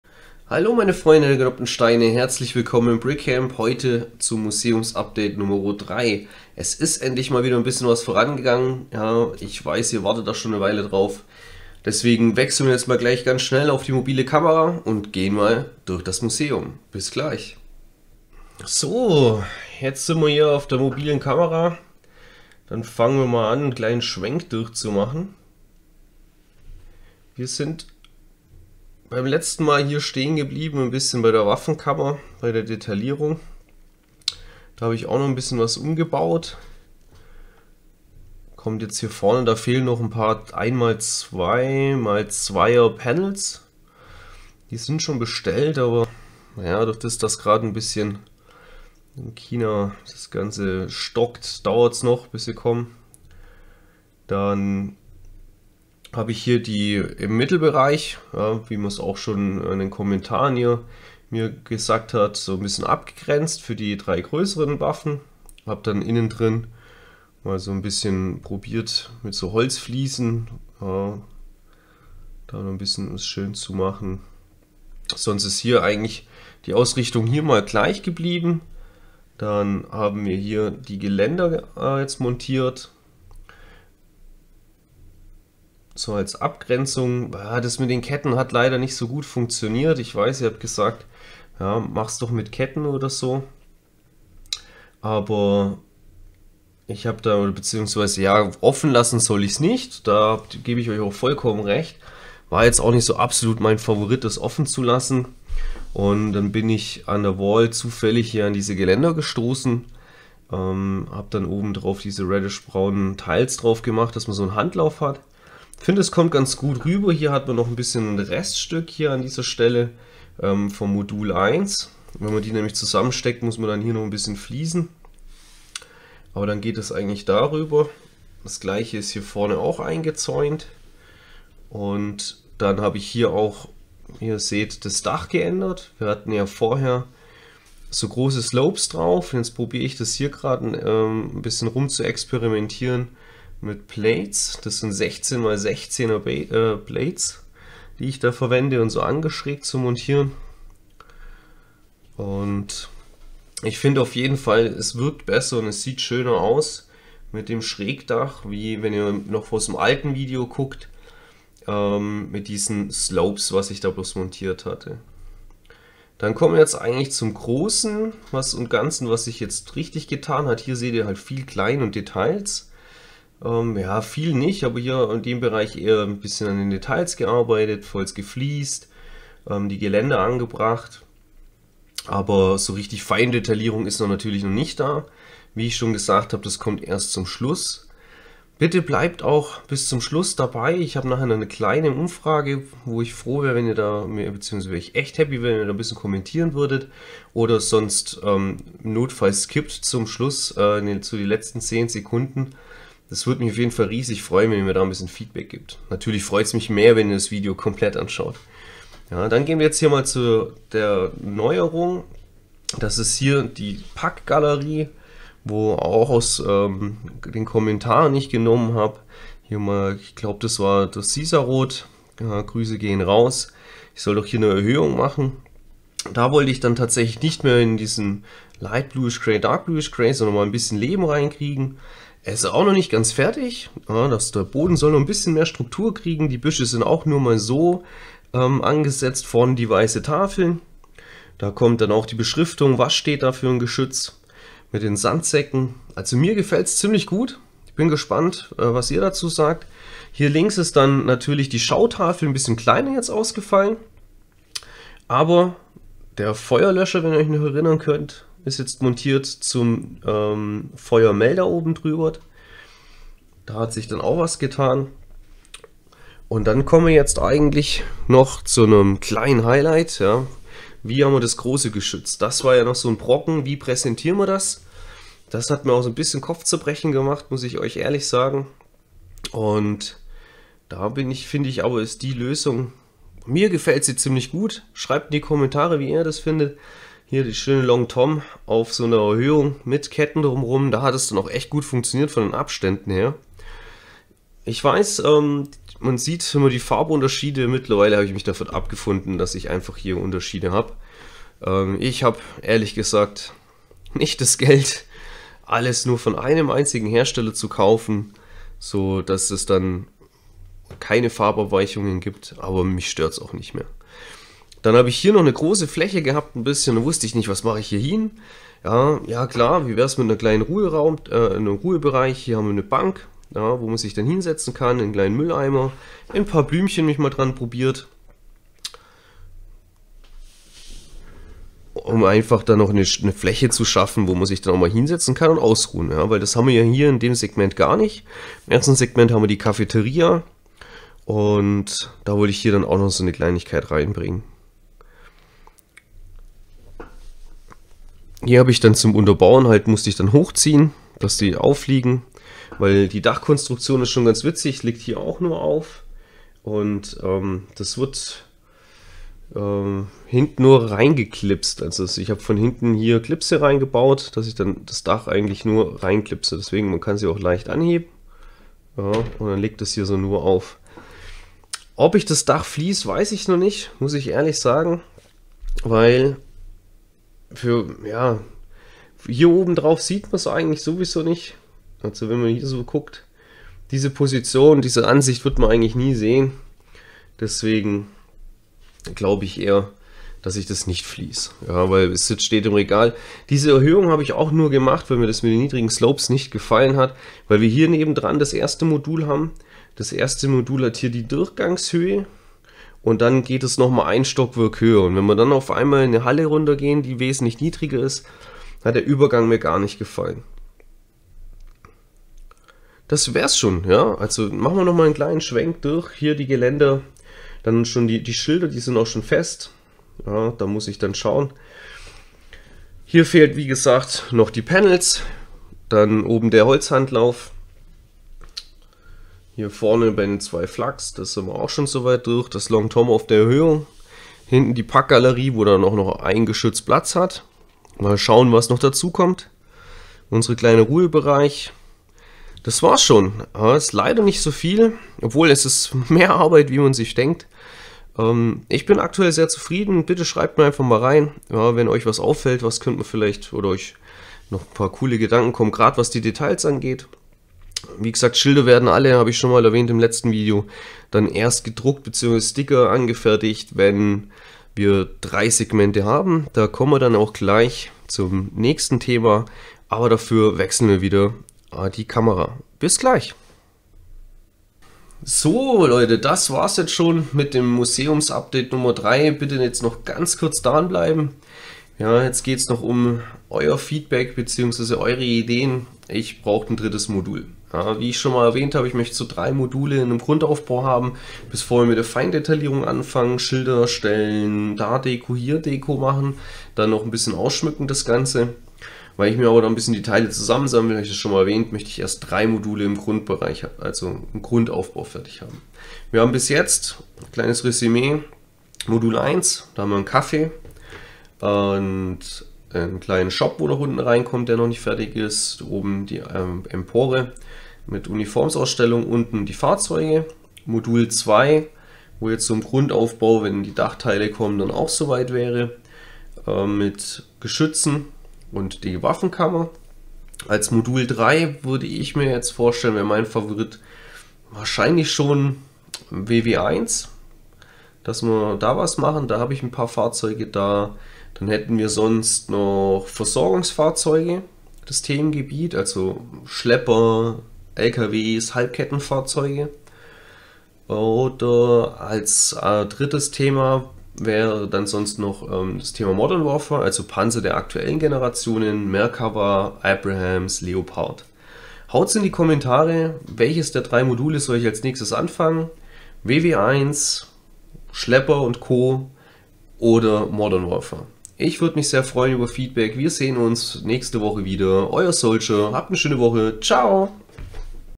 Brickmuseum Part 3 - Die Geschütze bekommen eine Heimat
Hi Leute

Endlich ist es mal wieder so weit es geht voran in meinem Brickmuseum.
Scháut rein was sich so getan und un bleibt bitte bis zum ende dran da ich mal wieder euren input brauche.

Viel spass beim Schaun wünscht euch euer Soldier!